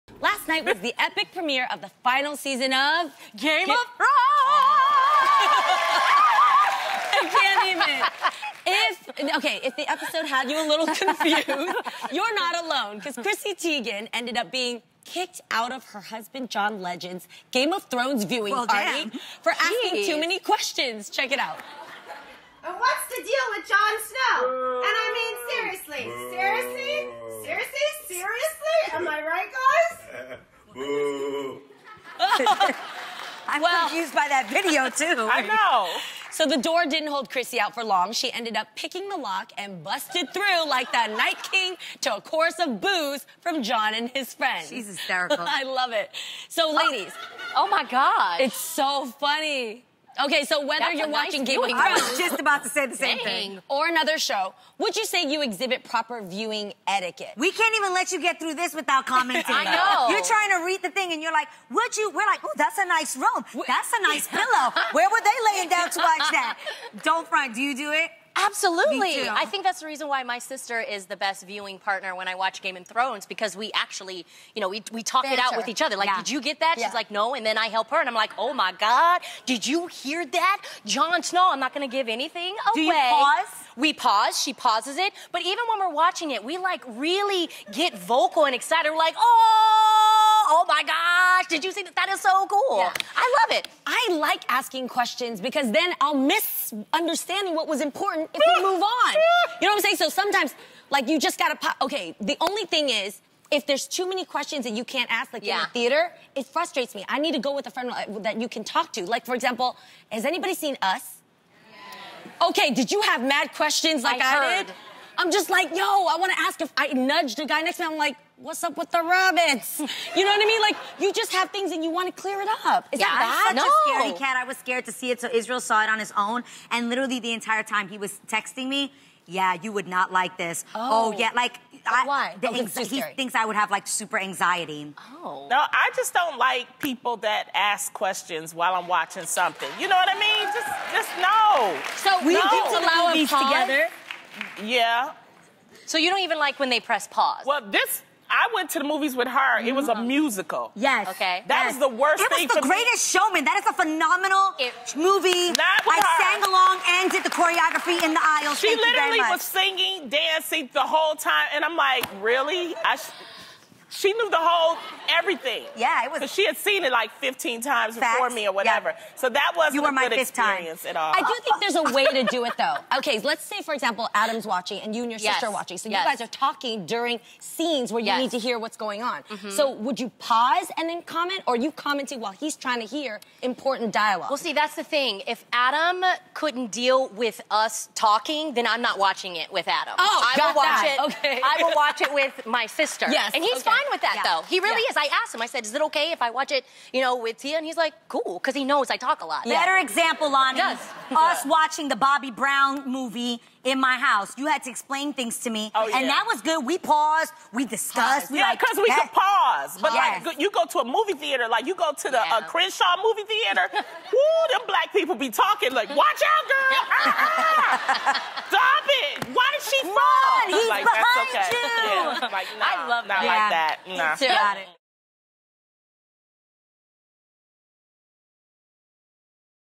Why Chrissy Teigen Was Kicked Out of John Legend’s 'Game of Thrones' Screening
Real fam, do you have proper viewing etiquette?